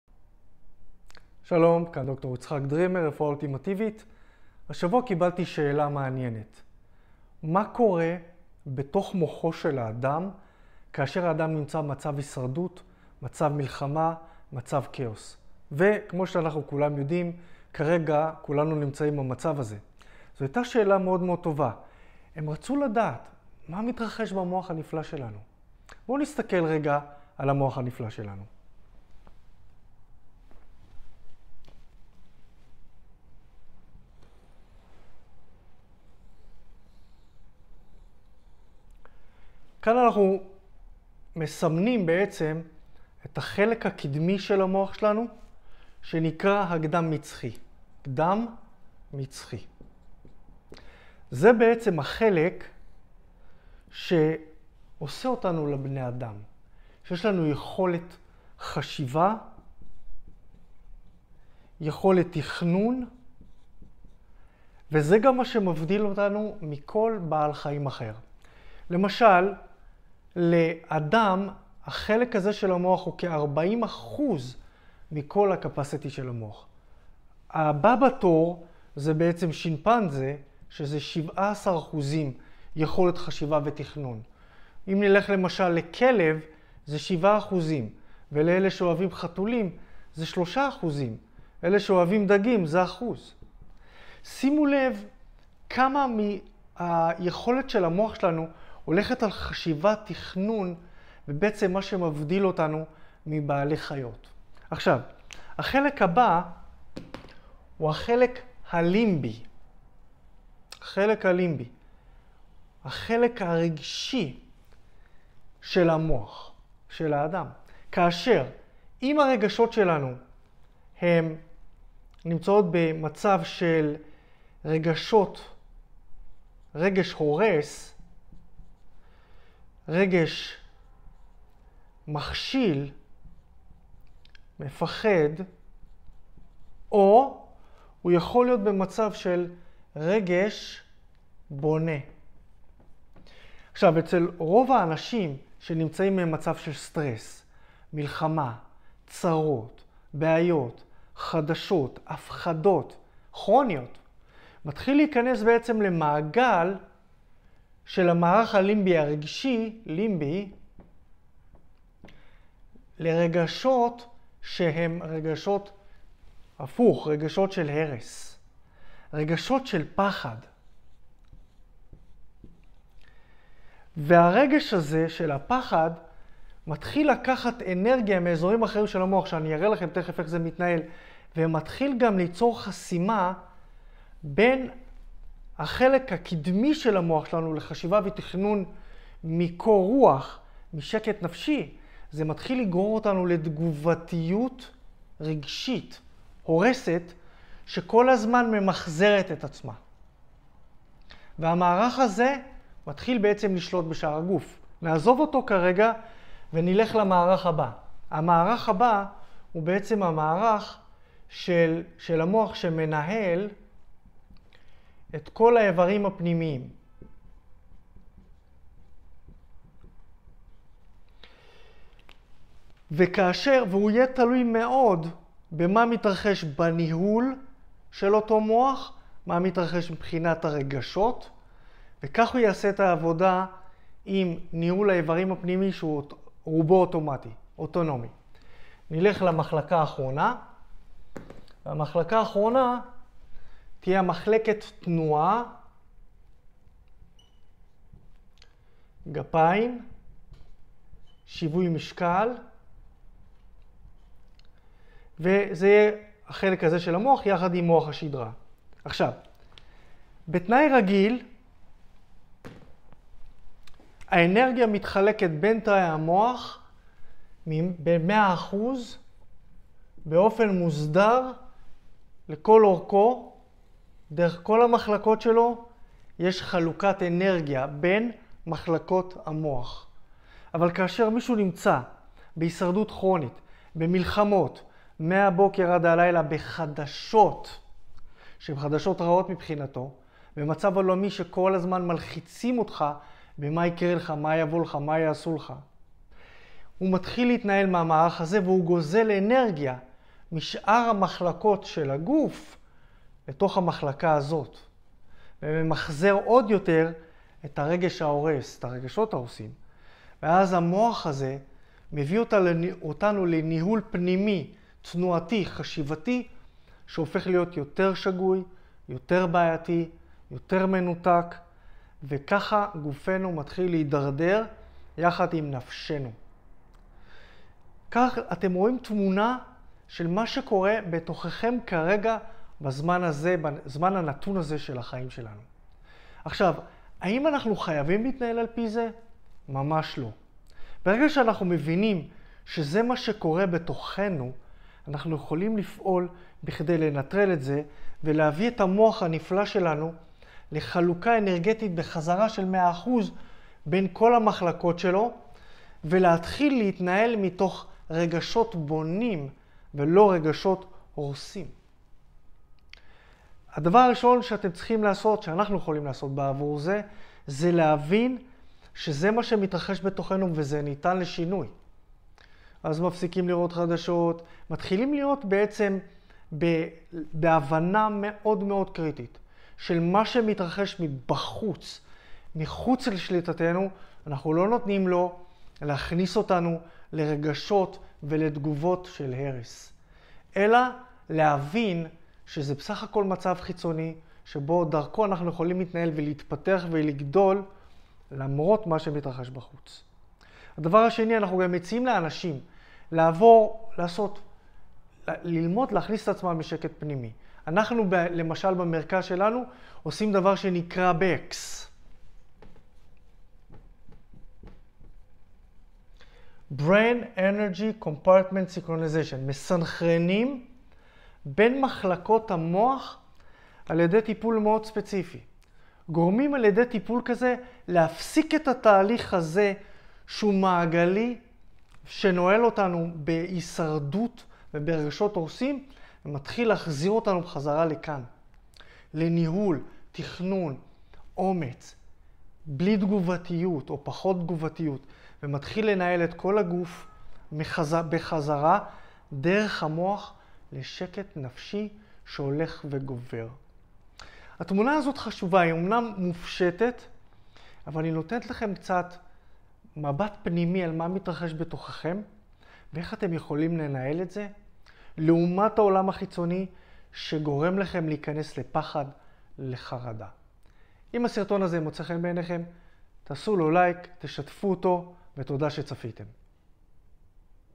מה קורה במוח שלנו במצבי הישרדות, מלחמה וכאוס?
בסרטון הזה ד"ר דרימר מסביר לעומק  איך האירועים שמסביבנו משפיעים על הדרך שבה המוח שלנו עובד. רוצים להבין מה באמת קורה במוח שלכם? צפו עכשיו!